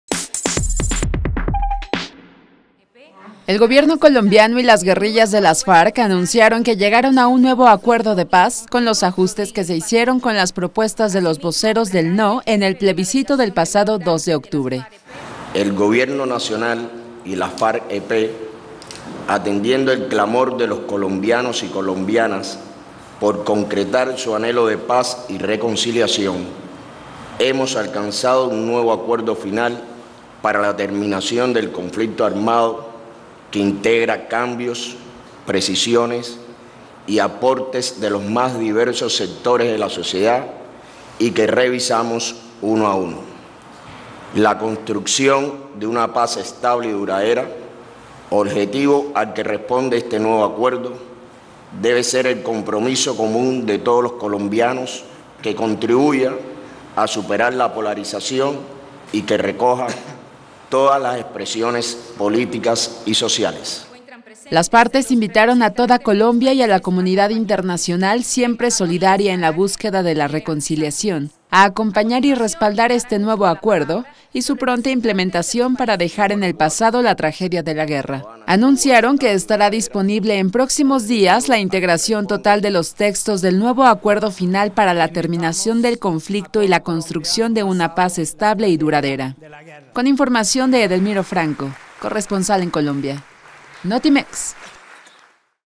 Gobierno de Santos y FARC logran nuevo acuerdo de paz
Colombianos y colombianas por concretar su anhelo de paz y reconciliación